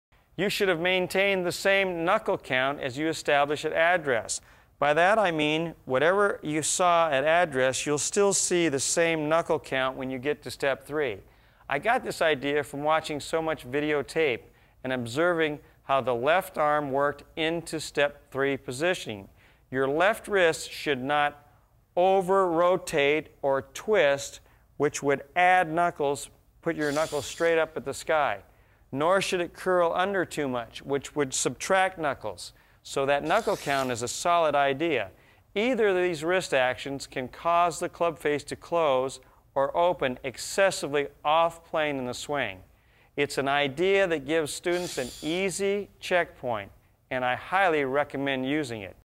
Lesson 3: The Knuckle Count: The Keys to Maintaining a Proper Knuckle Count
This video is from the 8 Step Golf Swing: #3 3/4 Backswing -- a new digital book that shows you with videos and text how to perfect your swing step by step, with expert golf pro Jim McLean.